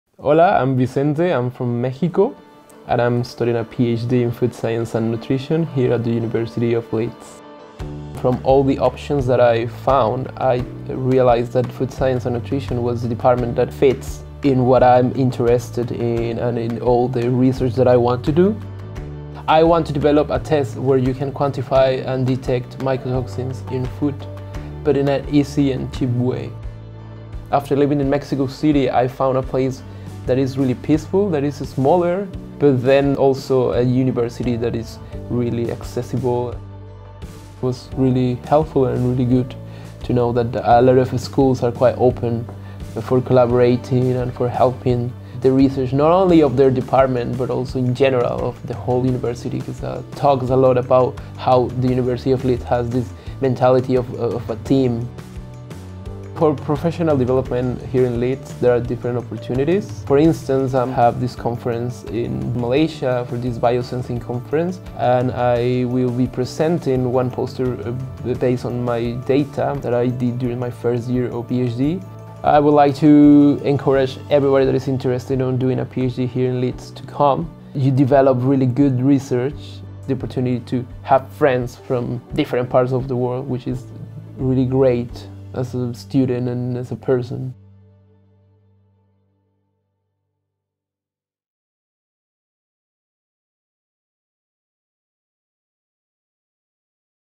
Study a PhD in the UK as an International Student at Leeds
A day in the life of a PhD student in Leeds means working with leading thinkers, pushing the boundaries of knowledge and contributing to research that matters.

Meet Vincente, a Mexican student studying a PhD in Food Science and Nutrition in the UK.

If you're considering a PhD in the UK, go further, come to Leeds.